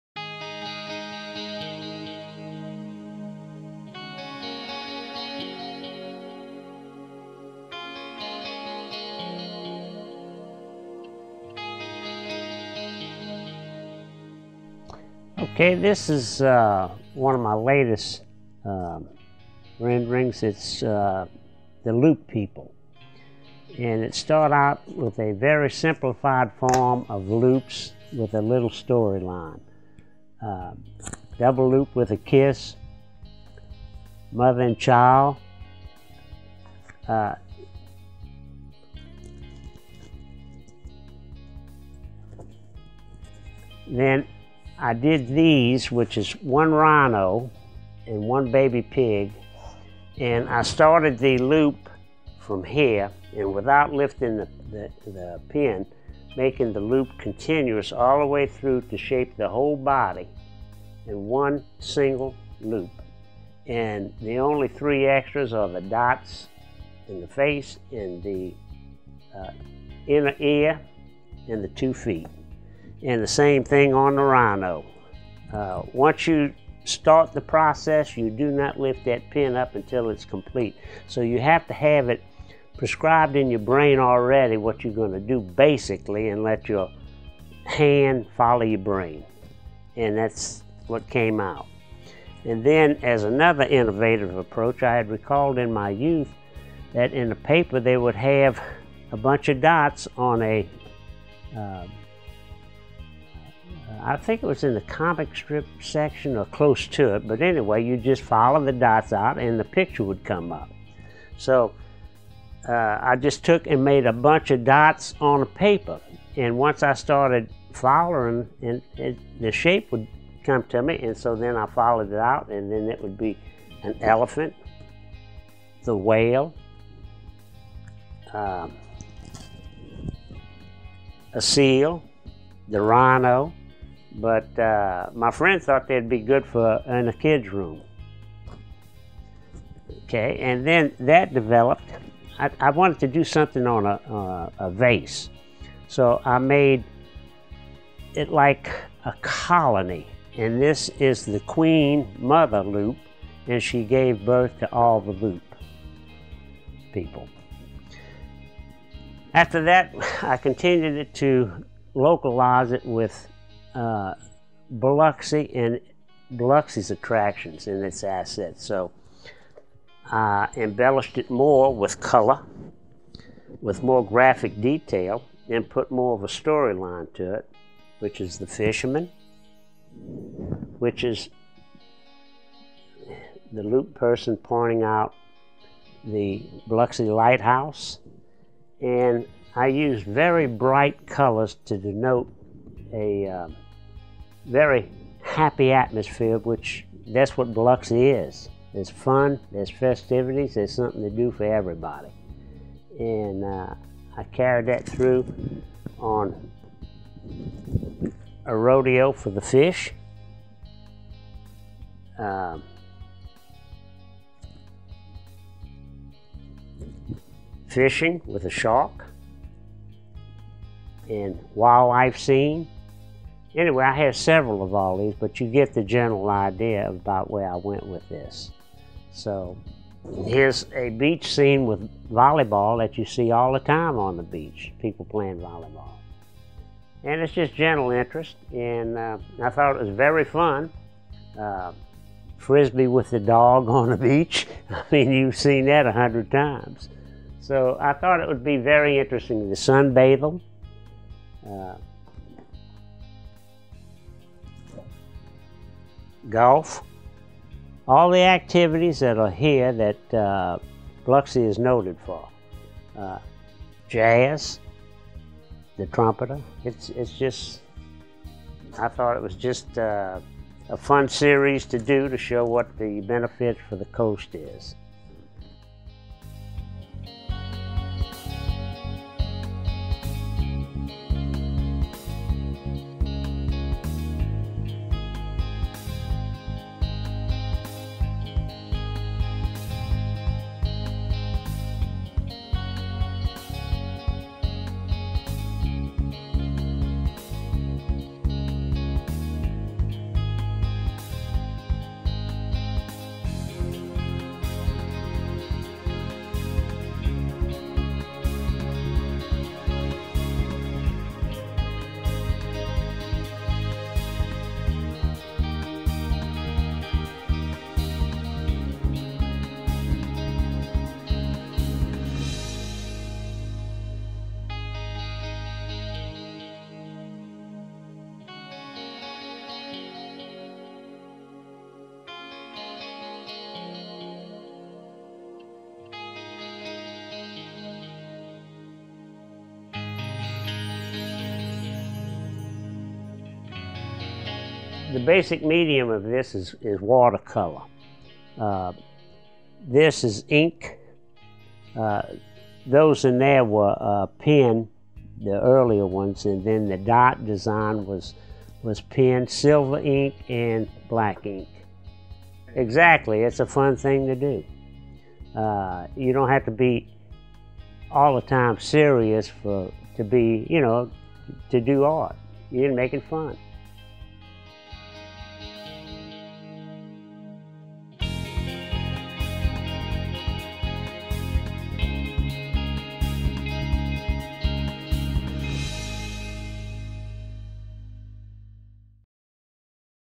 Wayne Rosetti - The Loop People
Seventy-six-year-old Wayne Rosetti has been painting since the age of twenty, producing over a thousand remarkable pieces.  This exclusive sampling of his life's work is a must-see, demonstrating his experimentation with multiple styles and media.  Inspirations from Van Gogh, Picasso, and Da Vinci, draw attention with their fetching golden colors and demanding images.  Religious influences with iconoclastic representations such as "Animal Kingdom" from the Bible and "Holy Man" mix with African-style sculptures.